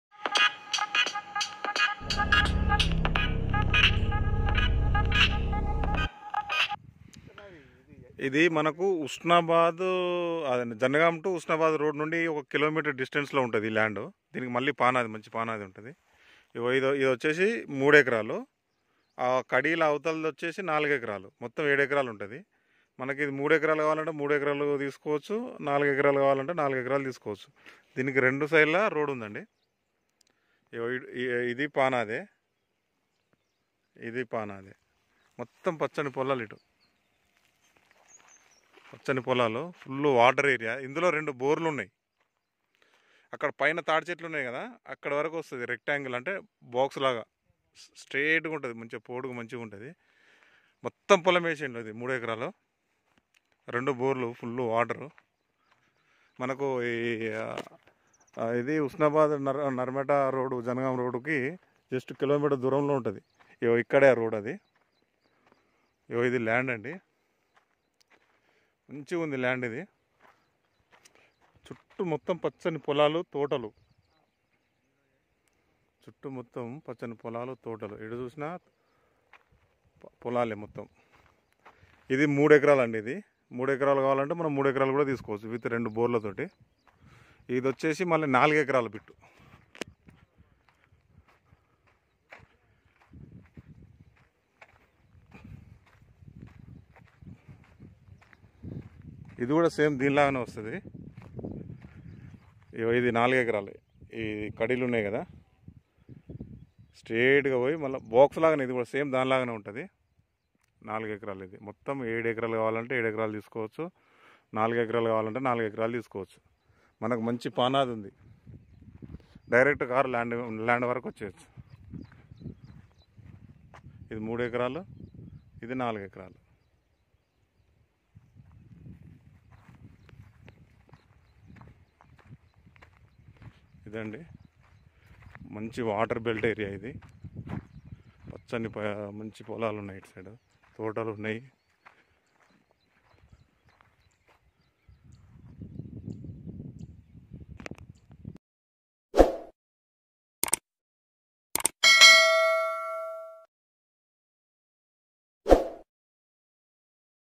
3 Acres polam ll 2 Bores ll 2 saides road ll jangaon district ll telangana ll
3 Acres polam ll 2 Bores ll 2 saides road ll jangaon district ll telangana ll

narmetta mandal town to 2kms
100 feet Road to 1km
Jangaon District to 20kms

pure red soil
per acre 27 lacks only

Clear title property
TS New pass book available
Raithu bandhu also done